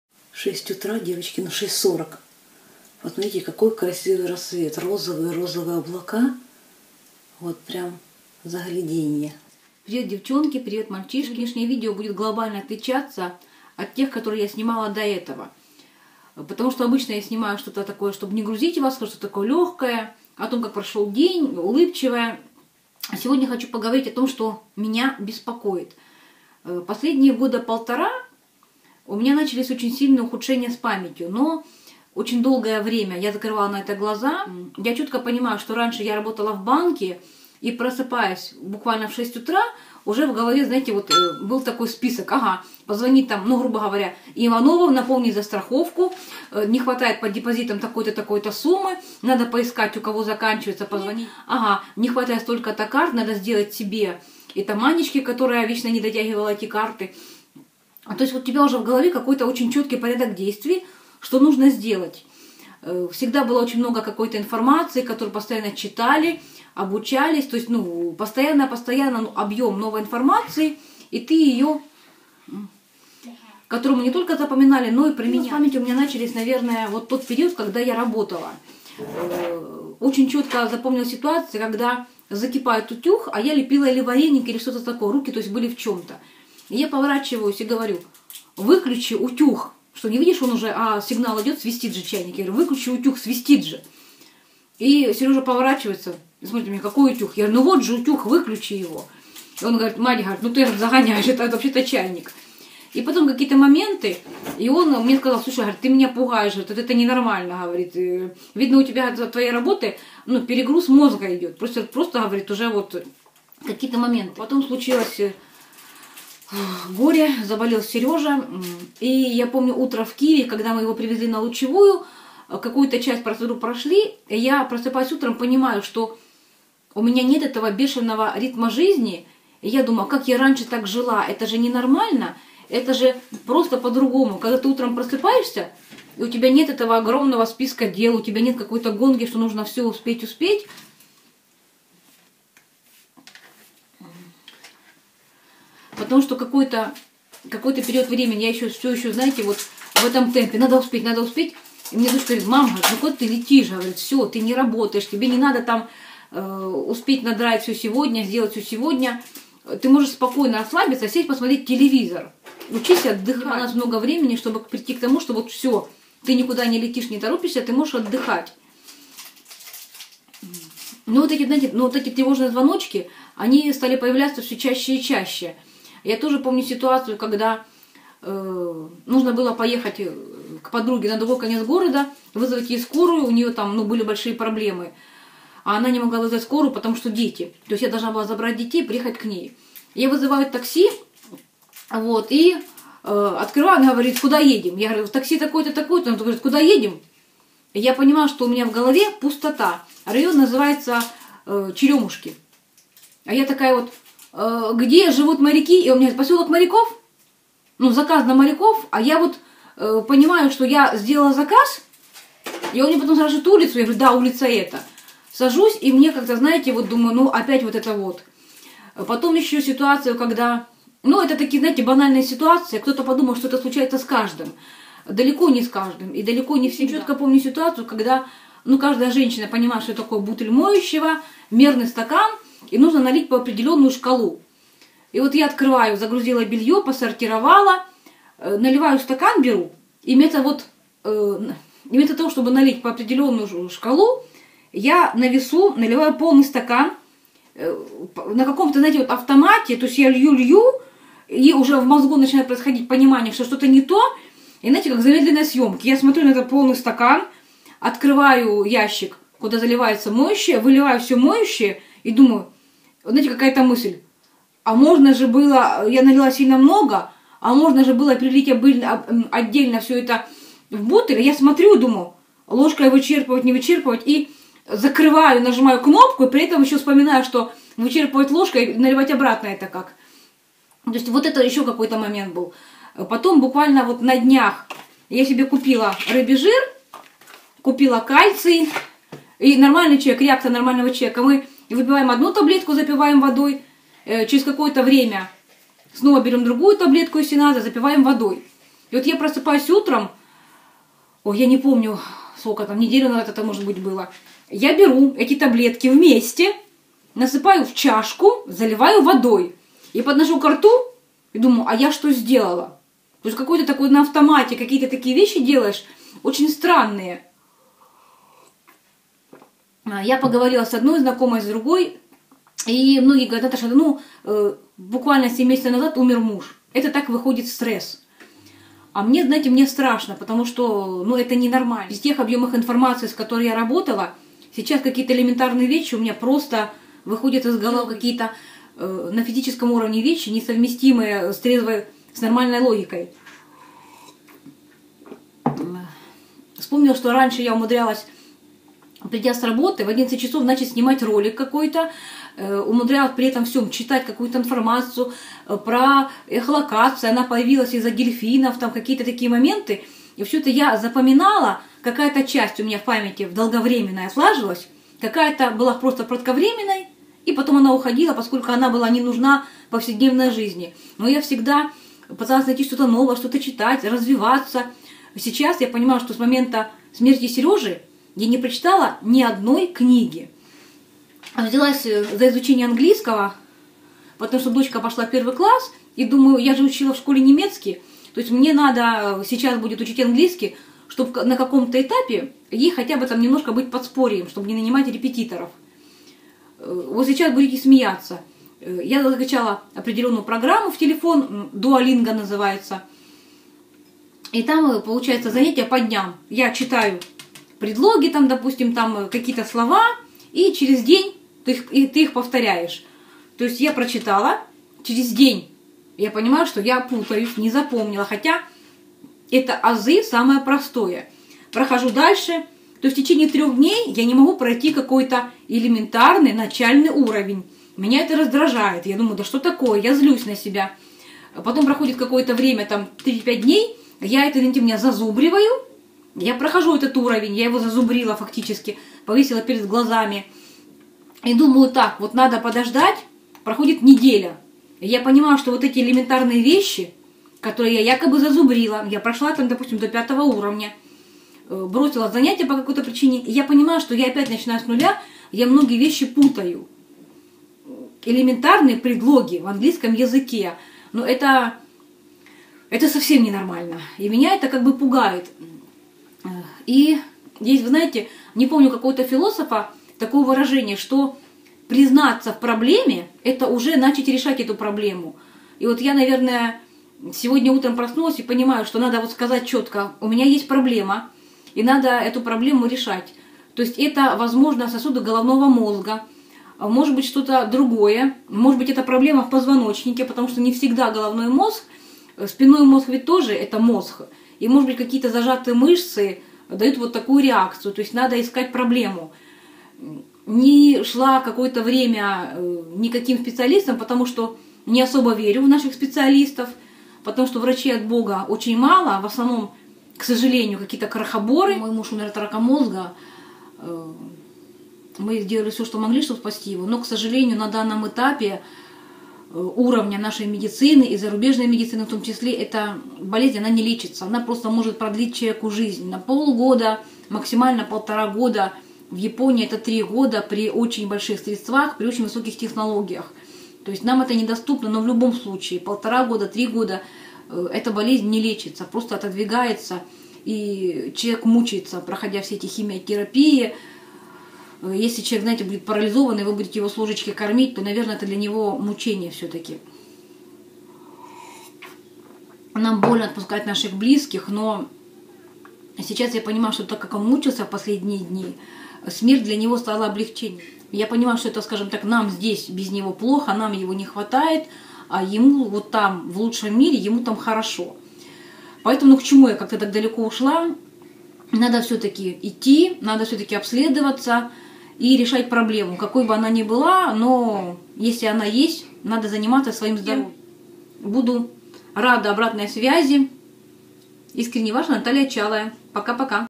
Проблемы с памятью - я обеспокоена,может ли стресс так влиять на память,начало болезни Альцгеймера?
Делюсь с вами своими переживаниями, за последние 1, 5 года ПРОБЛЕМЫ С ПАМЯТЬЮ и странное поведение в быту. Это при том, что долгие годы я работала в банковской сфере, у меня есть сайт, где все тексты написаны мной, в декретном отпуске я подрабатывала фрилансером (писала тексты на чужие сайты за финансовое вознагрождение). Это меня тревожит.